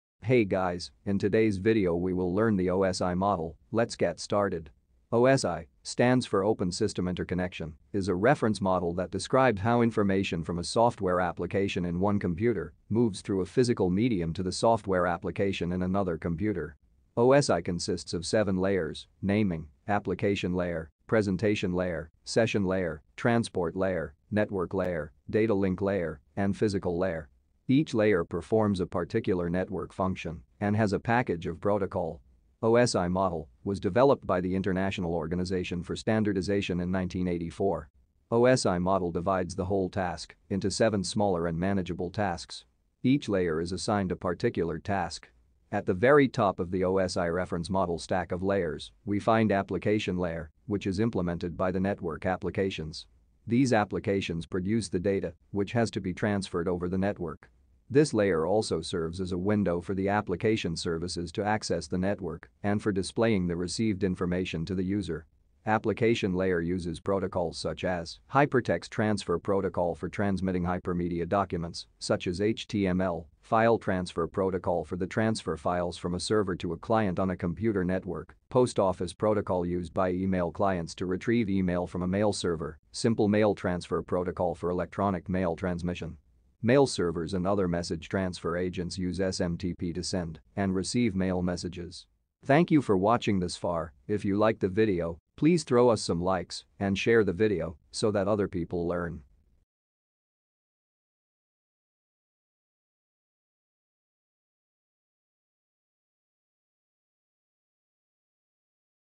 OSI Model (Part 1) - Application | Tech
Application layer is an abstraction layer that specifies the shared communications protocols and interface methods used by hosts in a communications network.

The application layer abstraction is used in both of the standard models of computer networking: the Internet Protocol Suite (TCP/IP) and the OSI model. Although both models use the same term for their respective highest level layer, the detailed definitions and purposes are different.